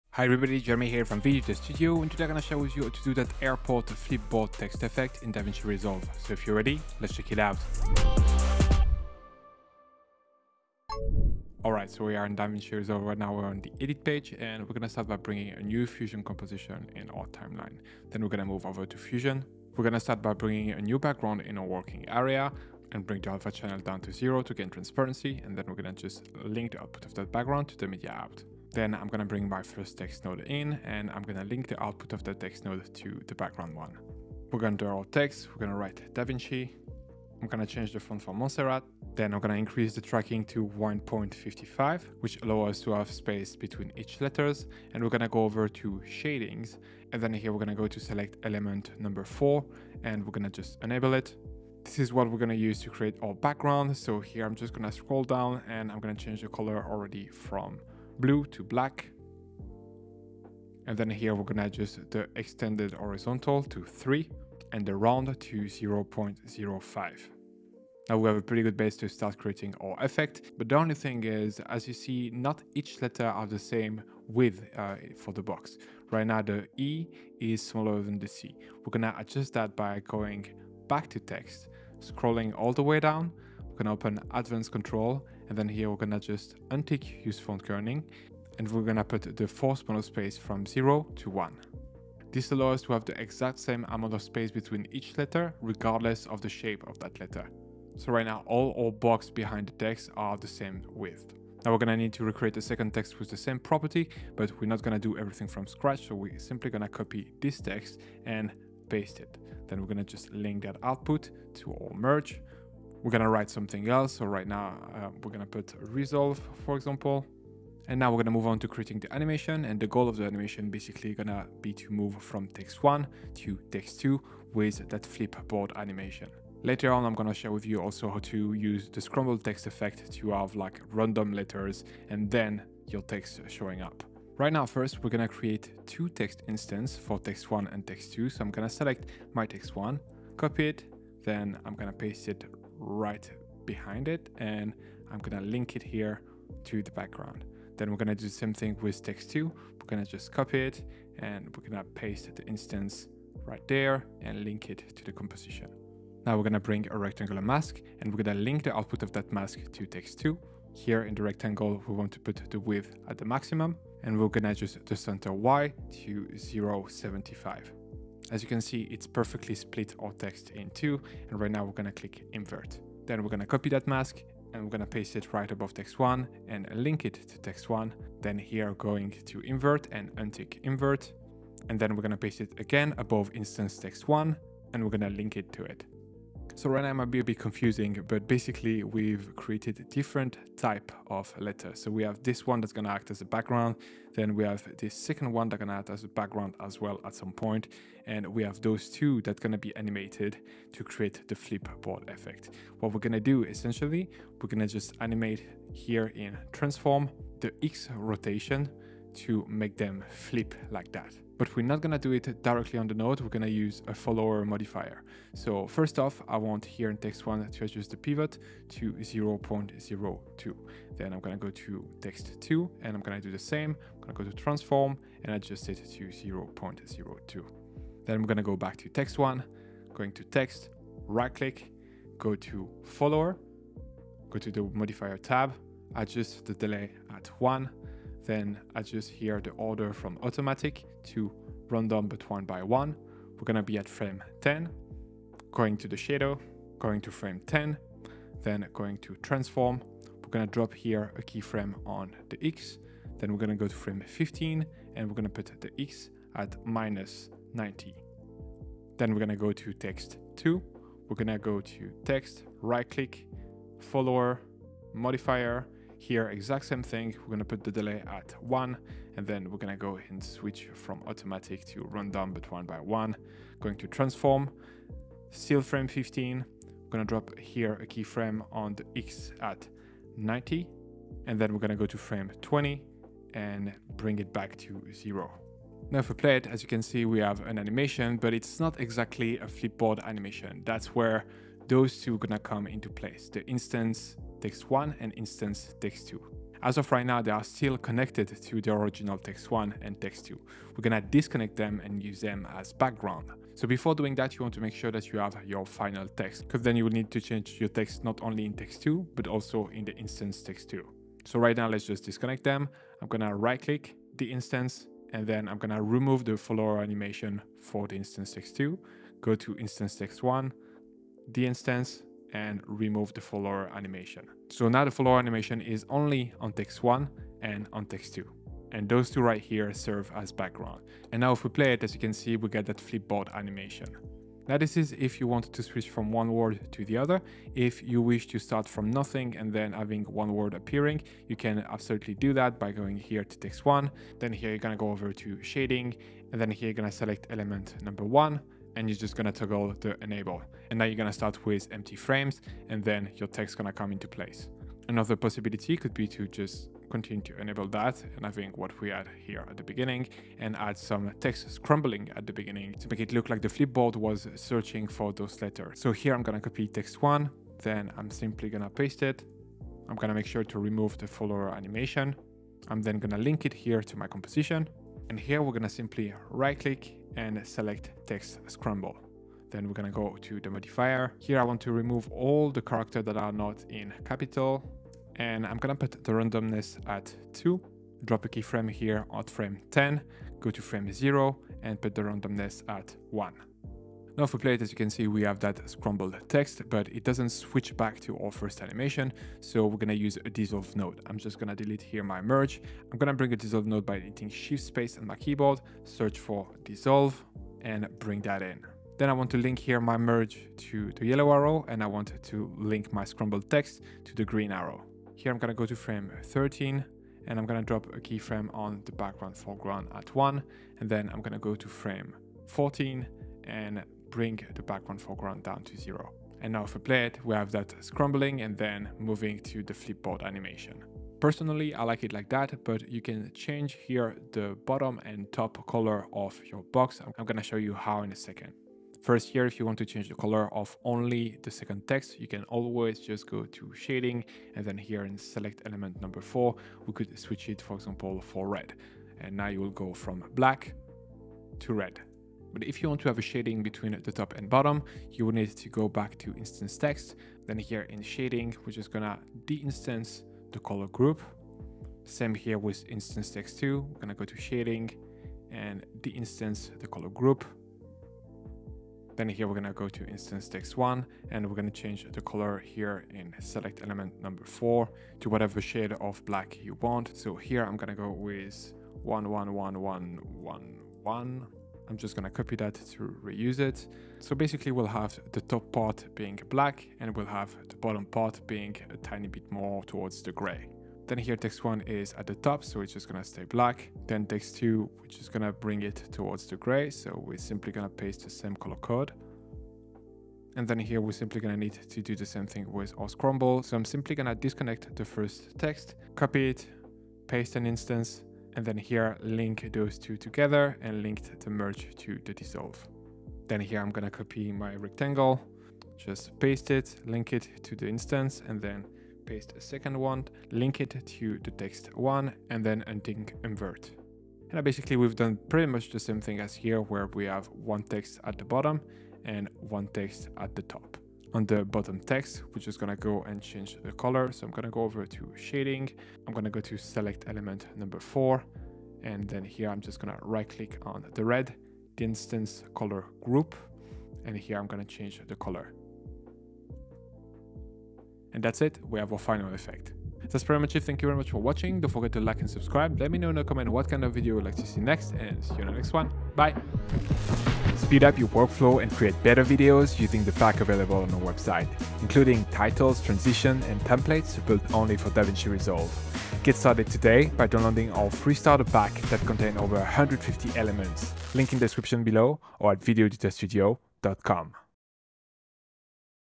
Airport Flip Board Effect in Davinci Resolve
In this Davinci resolve tutorial, I'll share with you how to do an airport flipboard text animation.

I've provided timestamps so you can easily jump between sections:

00:00 - Intro
00:10 - How to create text with letters inside individual boxes
01:45 - How to create the flipboard animation
05:35 - How to create scramble text flipboard animation
07:15 - Change the color of the boxes